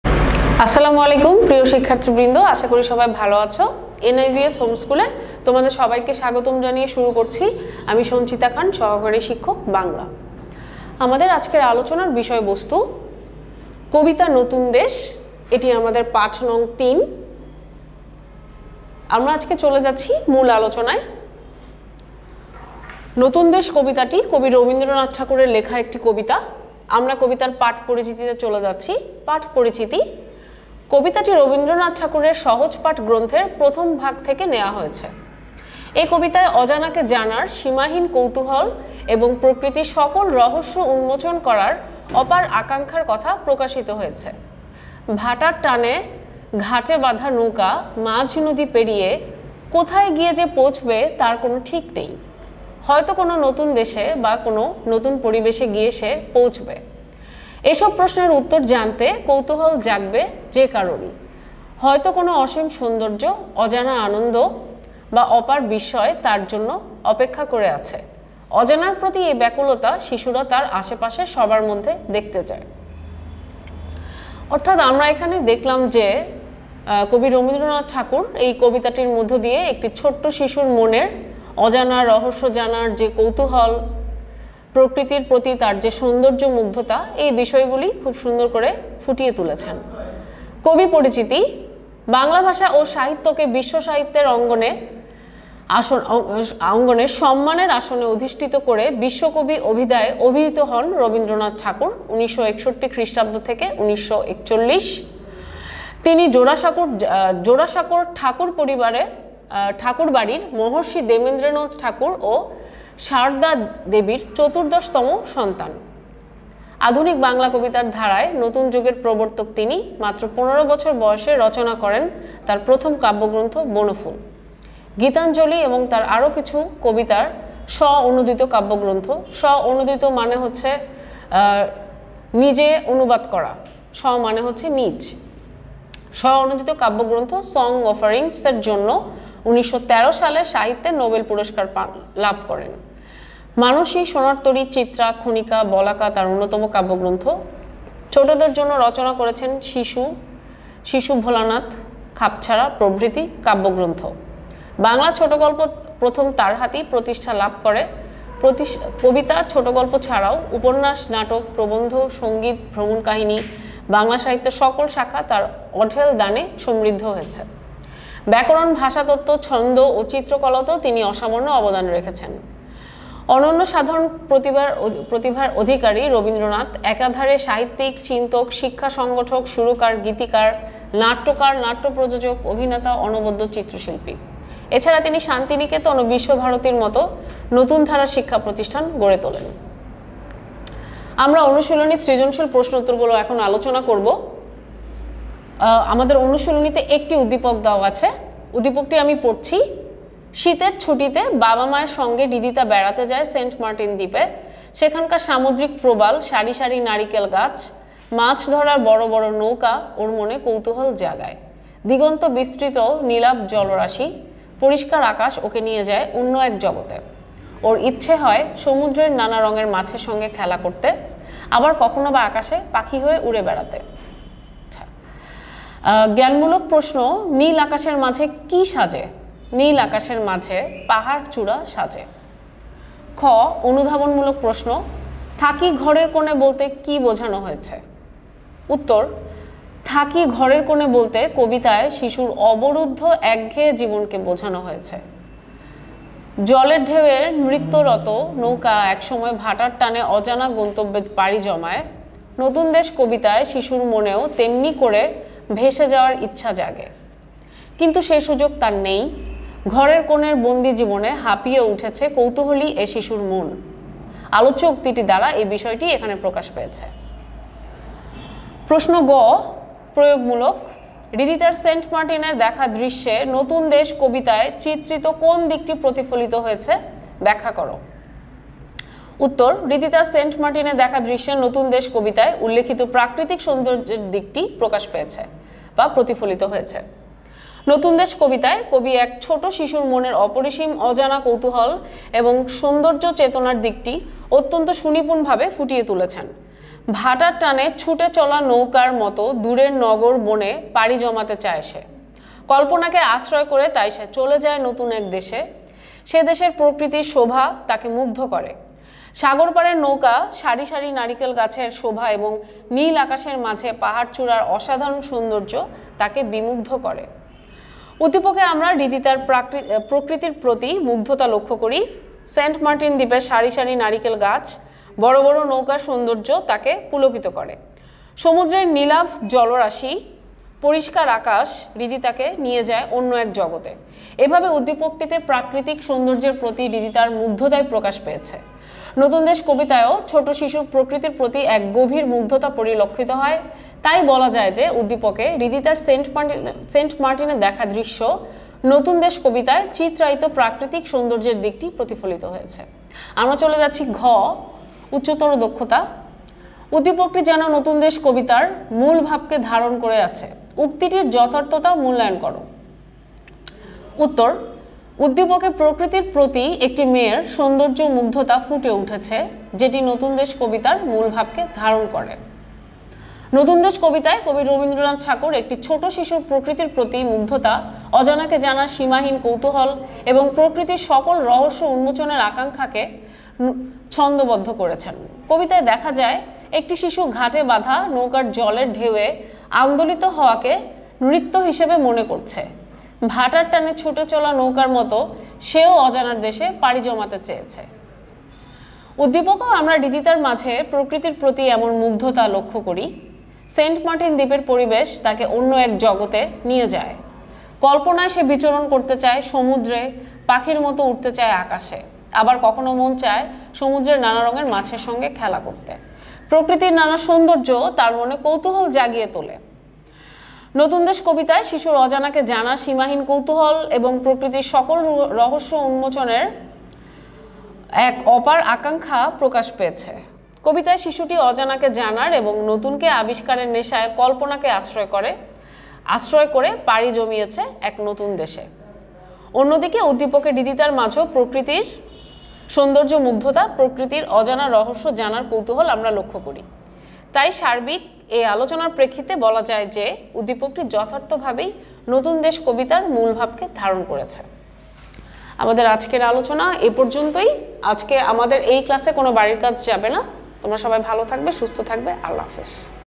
শ্রেণিঃ সপ্তম, বিষয়ঃ বাংলা ১ম পত্র (পাঠ- ০৩), আলোচ্য বিষয়ঃ নতুন দেশ (আলোচনা)
অর্ধ-বার্ষিক ক্লাস
শ্রেণিঃ সপ্তম
বিষয়ঃ বাংলা ১ম পত্র (পাঠ- ০৩)
আলোচ্য বিষয়ঃ নতুন দেশ (আলোচনা)
শিক্ষকঃ সঞ্চিতা খান